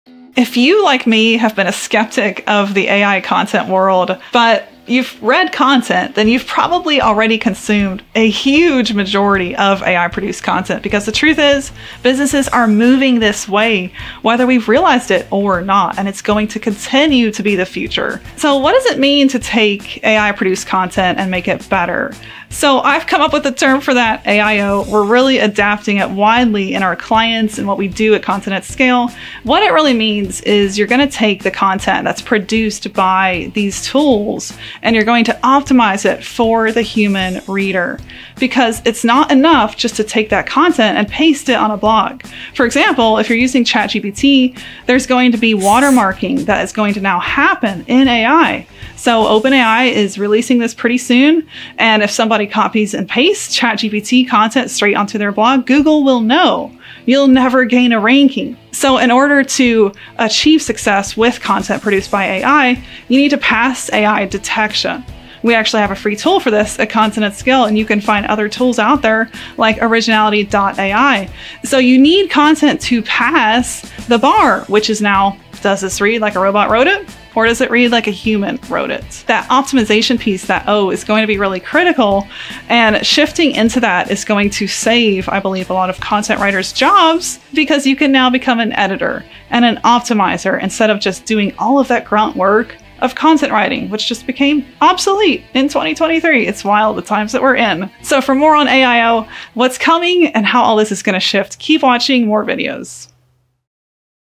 Is "AIO" the Next "SEO"?
Julia McCoy, VP of Marketing at Content At Scale, explains the meaning of Artificial Intelligence Optimization (AIO) and how it may soon impact much of the content you consume today.